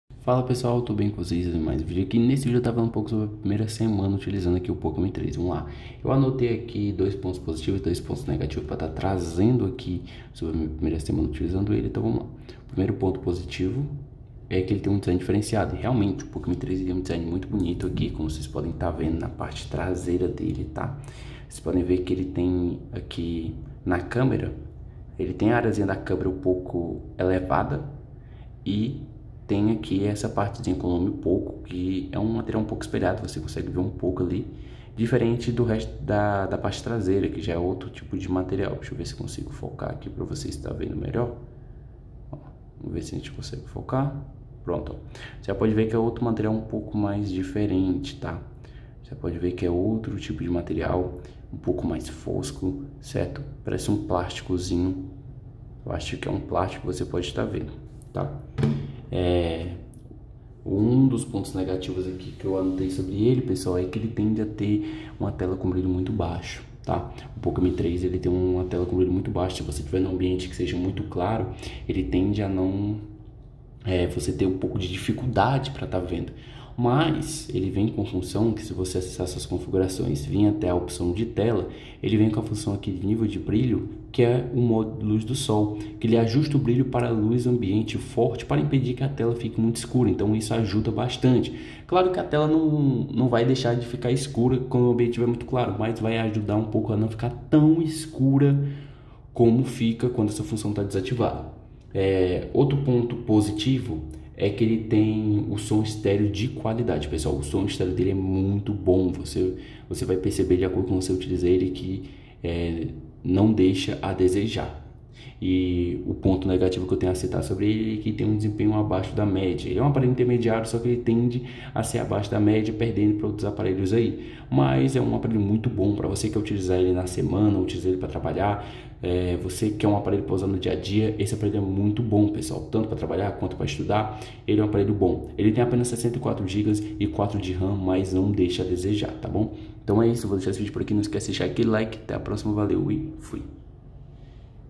Minha primeira semana usando o Poco M3
Fala pessoal!! vou mostrar nesse video oque eu achei sobre o Poco M3 na minha primeira semana de uso.

★ LINK ONDE COMPRAR:
★Moto G60